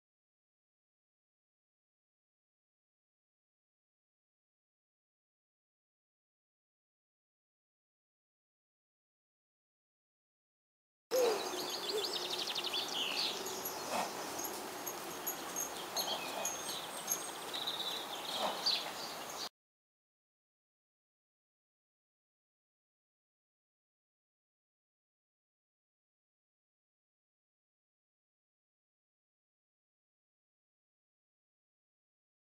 daisy.wmv
Homebred filly, born 2010, dam is KWPN by Kelvin ( Flemmingh x Ahorn lines ) sire is Copper King ( Rambo ).